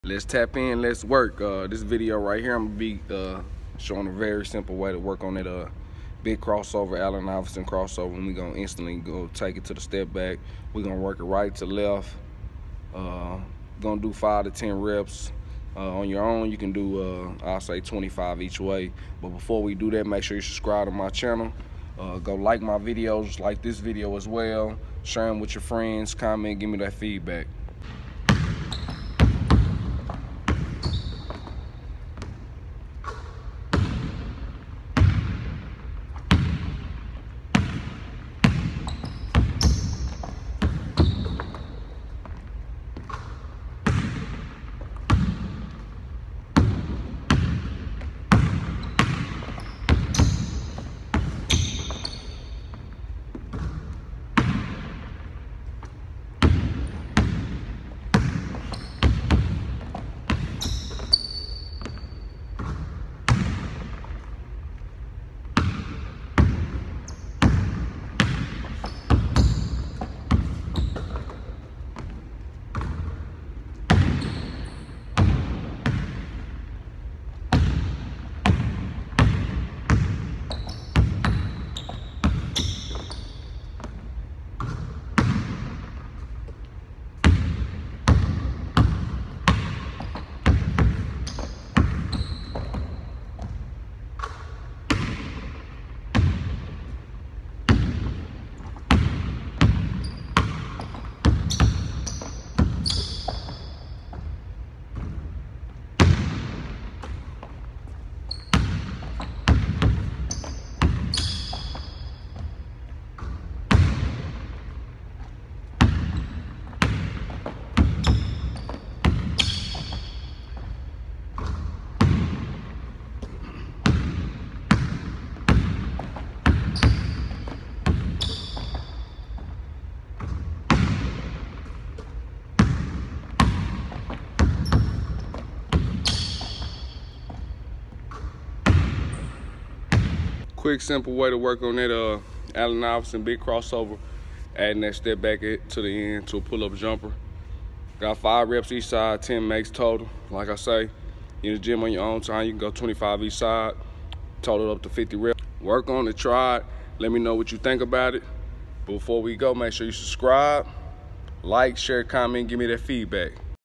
Basketball move Allen Iverson crossover to a step back jump shot
Working on our Allen Iverson crossover and finishing with a step back to a jump shot.